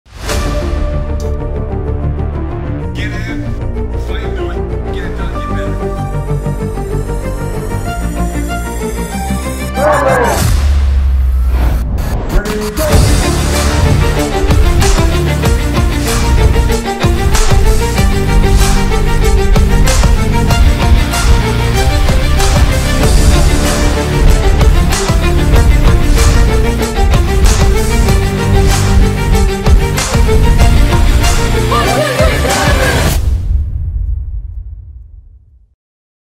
Master the game: The Ultimate Taylor Football Academy Experience
In this video, I teamed up with Taylor Football Academy to create a motivational & real scene of the training and hard work that goes into Taylors

Directed + Edited by Chance Darby

Official Channel of ByChance / Chance Darby